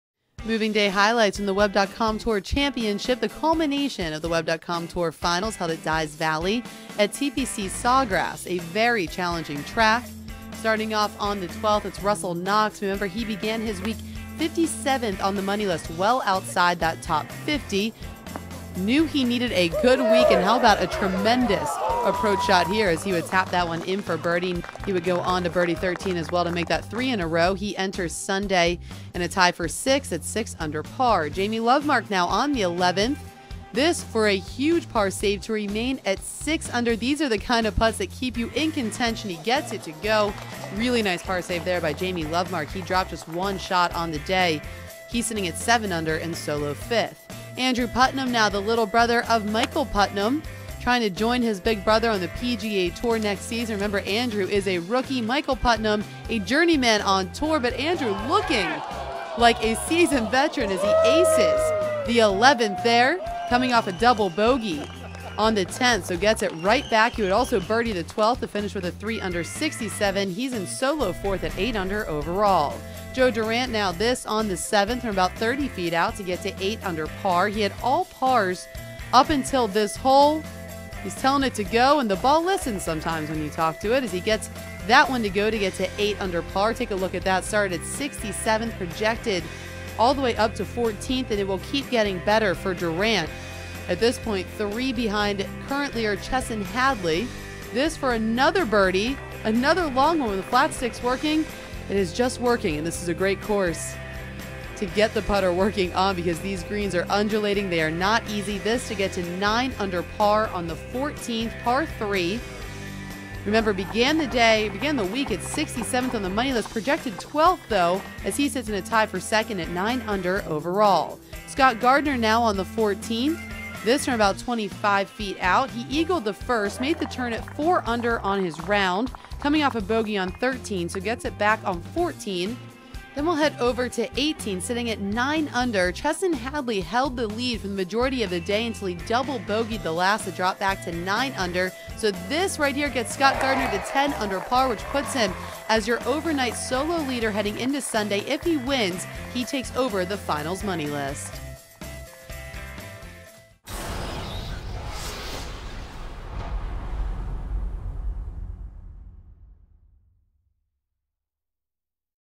Round 3 Recap: 2013 Web.com Tour Championship
In the third round of the Web.com Tour Championship on Dye's Valley Course at TPC Sawgrass, Scott Gardner leads alone at 10-under par total.